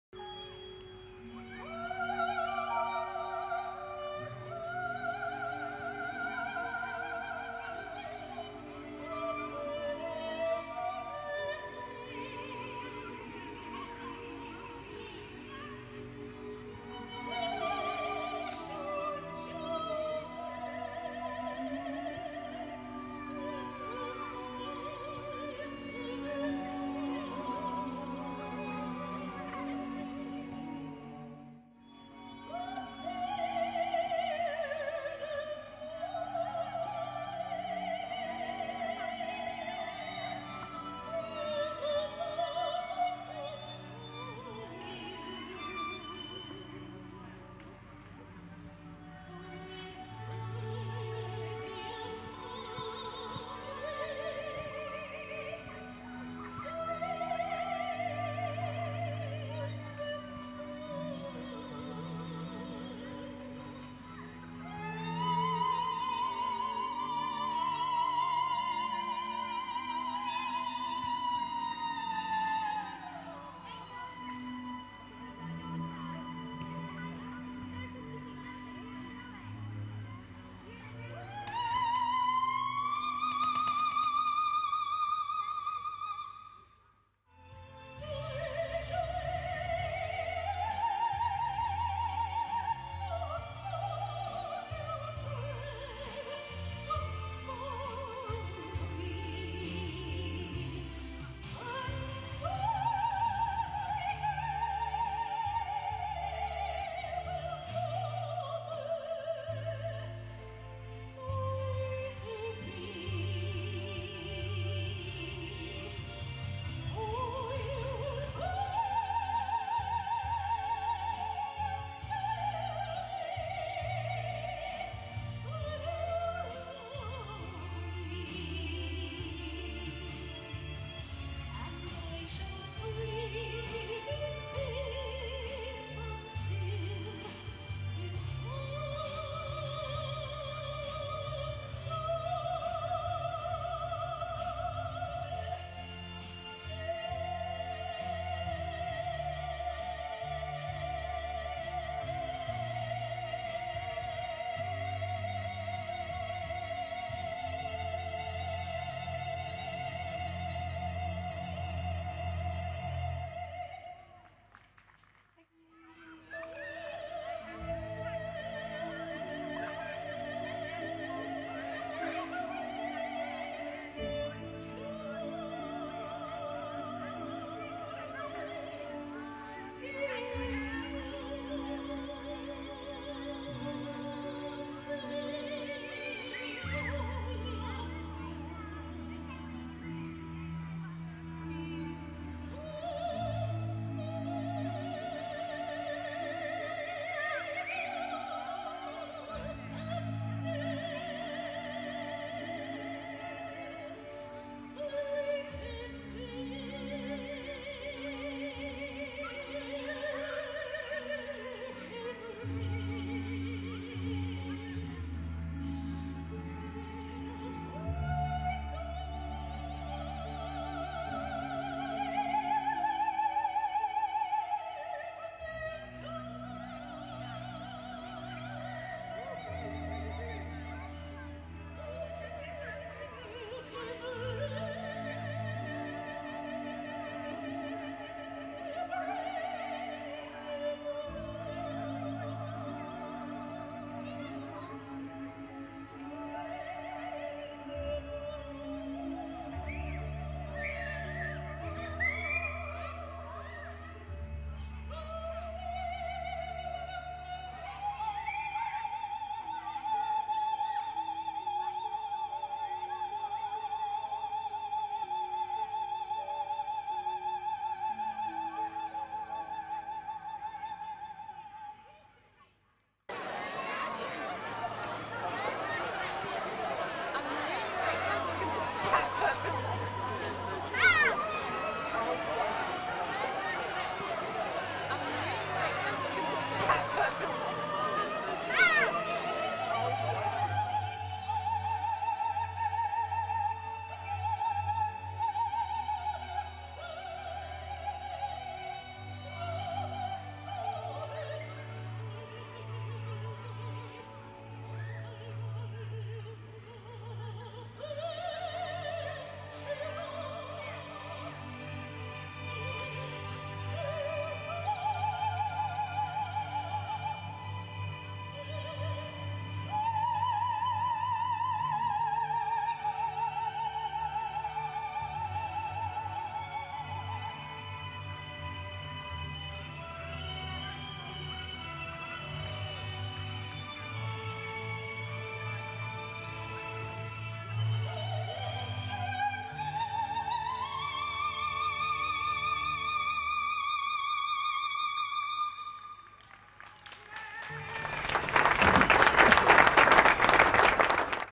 Claire Harvey East - Crossover Showreel 2016
Recorded live at "Proms in the Park"
West Bridgford Armed Forces Day - 25th June 2016